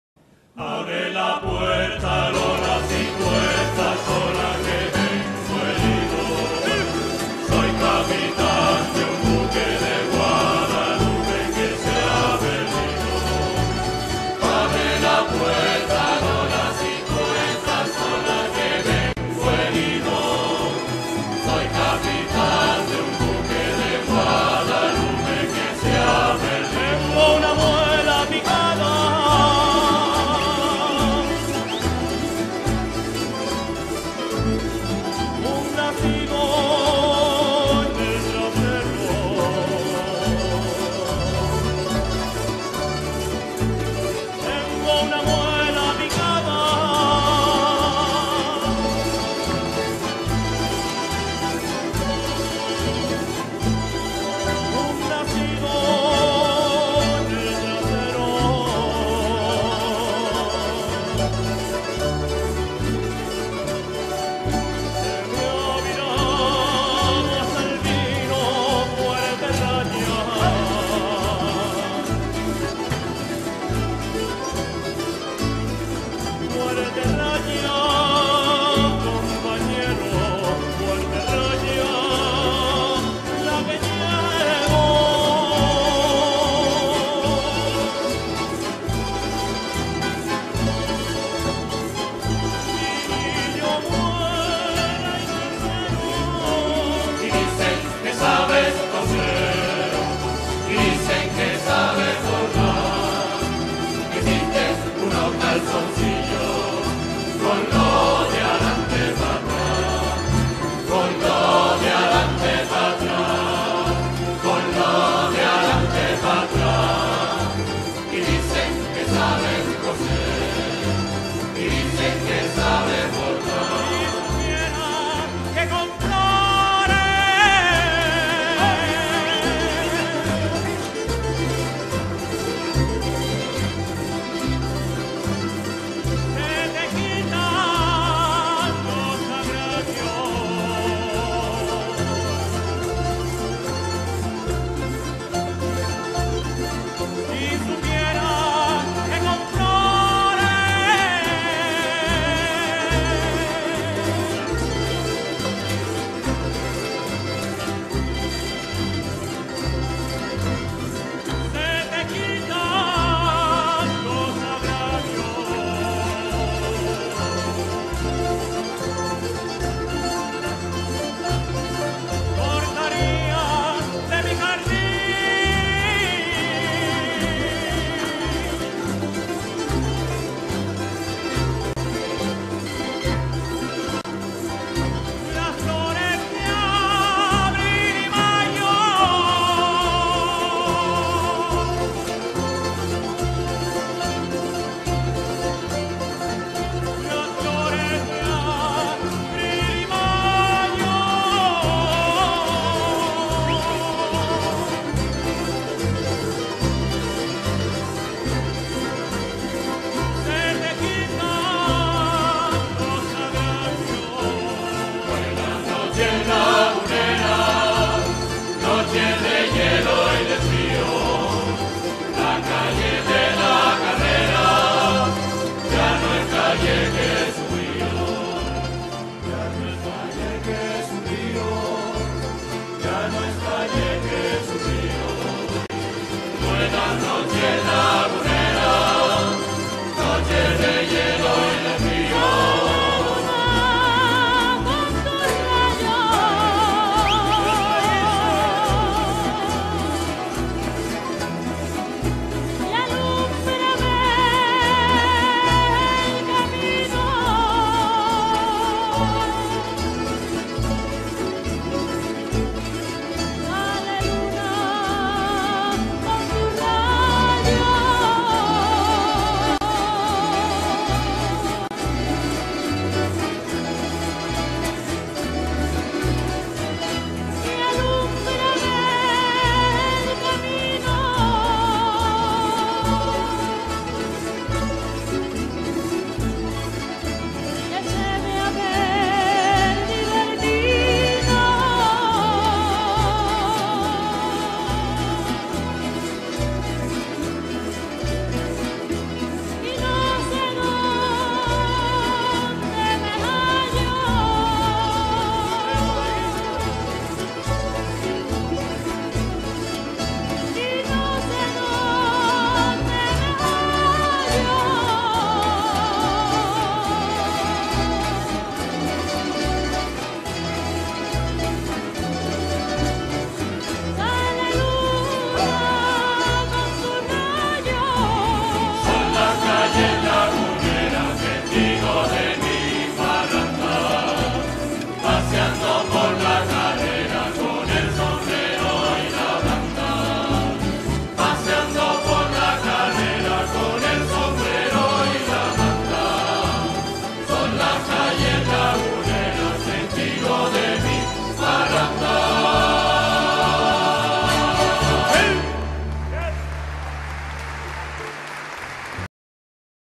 XXIX ANIVERSARIO PRINCESA IRAYA
29 años cumple la Agrupación Folclórica Princesa Iraya, en esta celebración se otorgó el Galardón Princesa Iraya a Alfredo Ayala Ojeda. Año 2011.